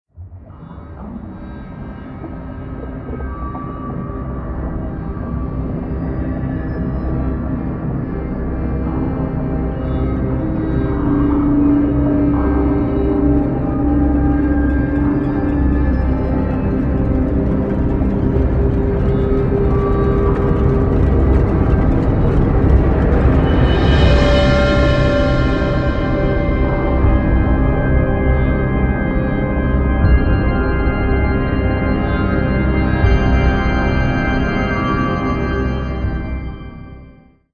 360 VR Animated Art
A piece of 360 VR animated art by A K Svenne, best viewed while wearing a VR headset.